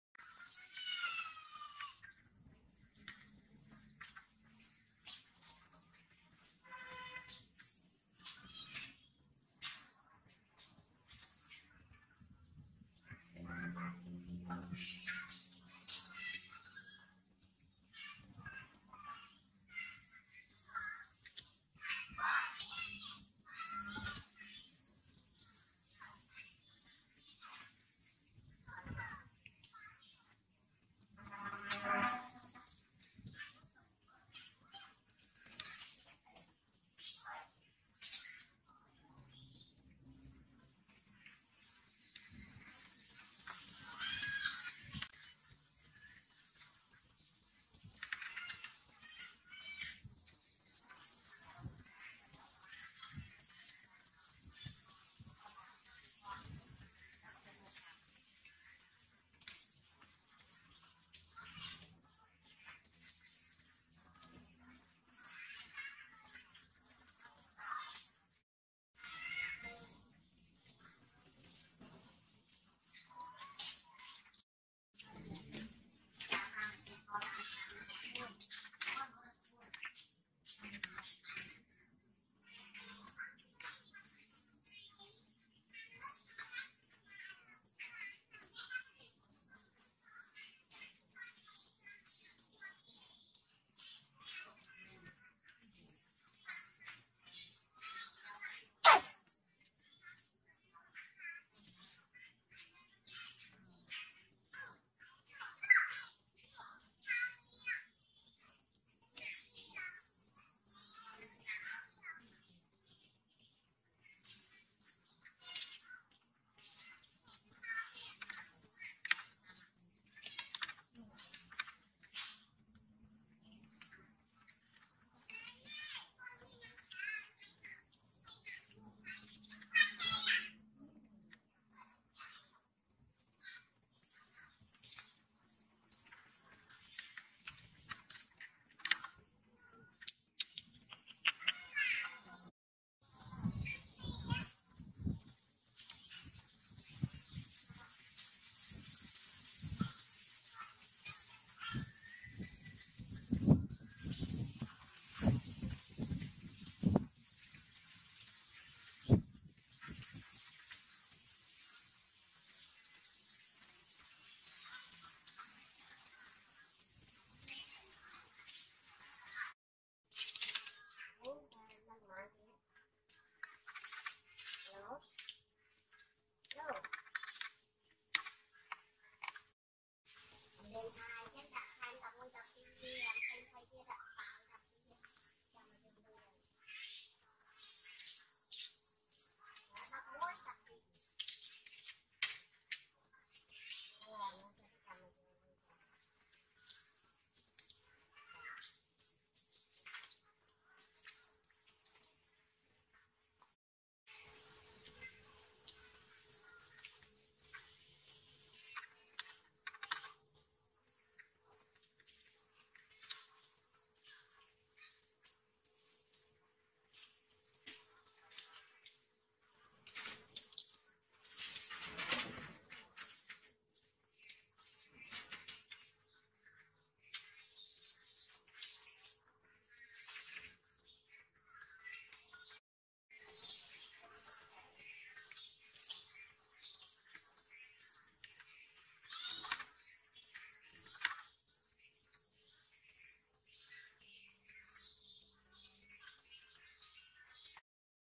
Very Pitiful Newborn Baby Slila don't get milk - Poorest newborn baby monkey - Primitive channel
Hi today i want to show you about video:Very Pitiful Newborn Baby Kathy don't get milk - Poorest newborn baby monkey

Primitive STA